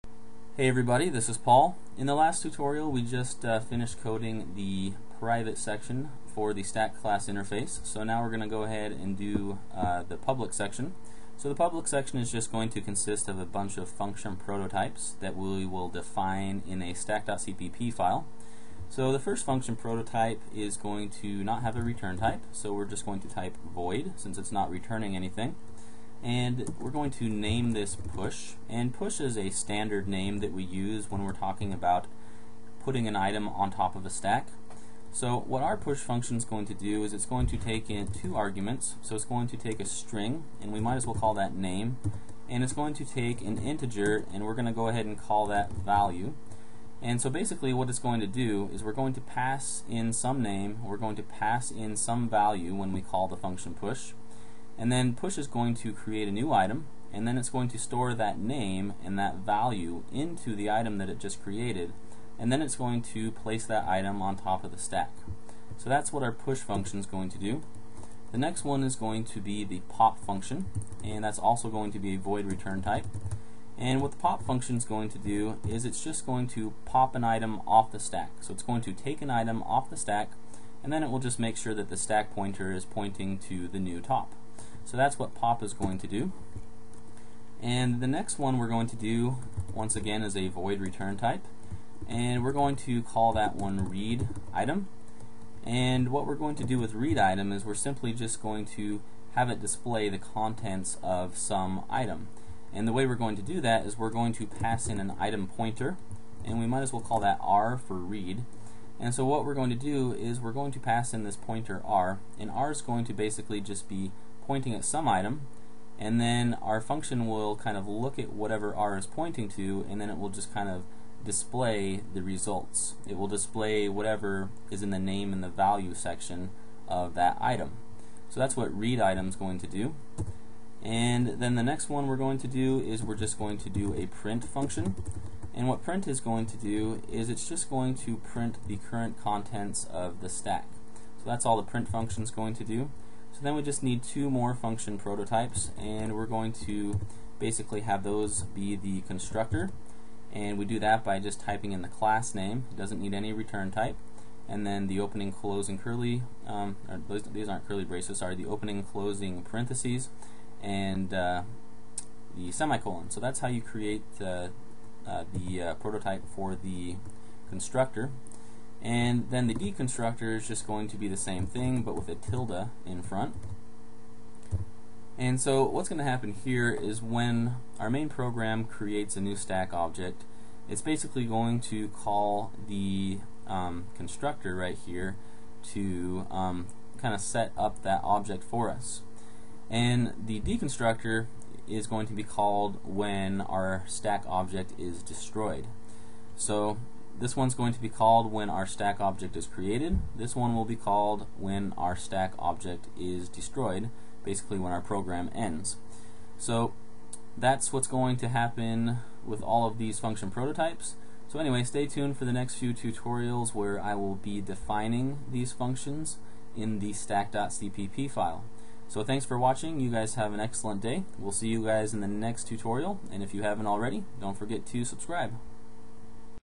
How to Create a Stack Class Project - ( Part 3 ) - Defining the Public Section of the Class
In this video I code the public section of the stack class.

Want to learn C++? I highly recommend this book

STILL NEED MORE HELP?